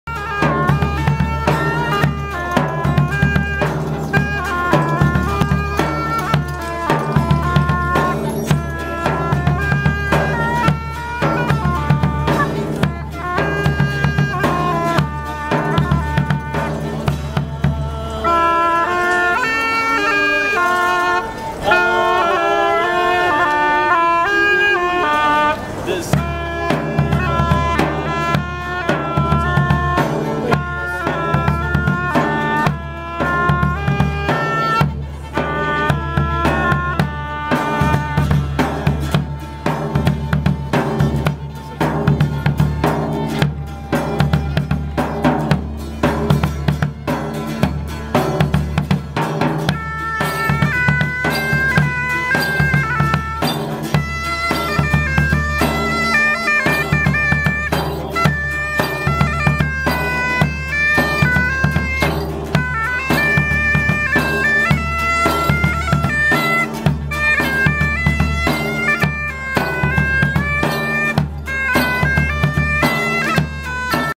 中世のマーケットで楽器を演奏する人たち 2015 0826
ZOMで行われたMittelalterspektakel （中世の光景）のマーケットの入り口で楽器を演奏する人たち
2015年8月26日　スイス　ZOM(Zürcher Oberland Messe)で撮影
All articles, photos,videos are NOT allowed to be used on other website,media without permission.
Please contact  if you want to use the video.